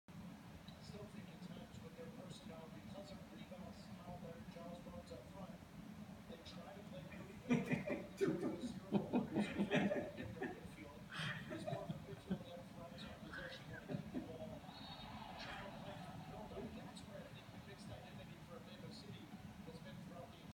Animalsdt cat chasing soccer ball on tv screen
cat chasing soccer ball on tv screen